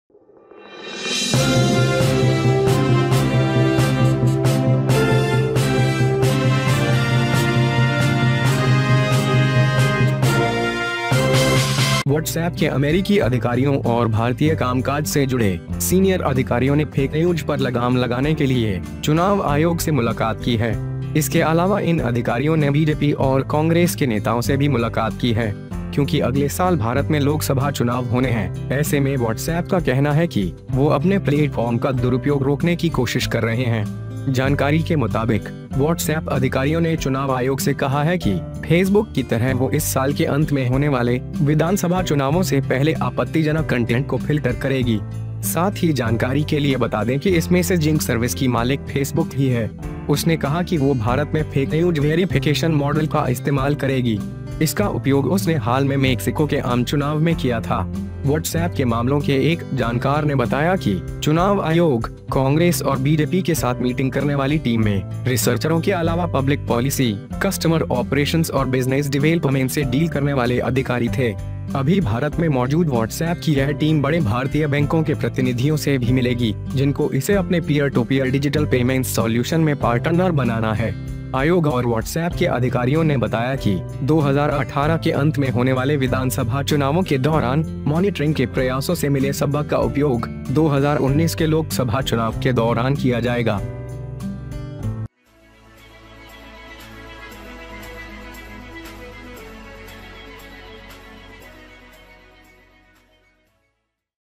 बड़ी खबर : Whatsapp का बड़ा फैसला, जरूर देखे...
Whatsapp ने चुनाव आयोग से किया फेक न्यूज पर लगाम का वादा, BJP कांग्रेस नेताओं से की मुलाकात...

Senior officials associated with US officials of Whatsapp and Indian operations, have met the Election Commission to rein in the Fake News.
Apart from this, these officials have also met BJP and Congress leaders. Because Lok Sabha elections are due in India next year, in such a way, WhatsApp says that they are trying to stop misuse of their platform. According to the information, the WhatsAppAppe officials have told the Election Commission that, like Facebook, they will filter out the 'objectionable content' before the assembly elections, which will be held later this year.
Also, for information, let us know that this messaging service owner is a Facebook user. He said that he would use the Fake News Verification model in India. He used it recently in the Mexican general election. One knowledge of Whatsapp case told that the team that was meeting with the Election Commission, Congress and BJP, apart from the researchers, was the executing officer with public policy, customer operations and business development.
This India-based WhatsApp company, which is present in India, will also be meeting representatives of big Indian banks who make it a partner in its Peer to Peer Digital Payments Solution. Commission and Whatsapp officials told that during the assembly elections to be held in the end of 2018, the lessons learned from the efforts of monitoring will be used during the 2019 Lok Sabha elections.

HJ NEWS !!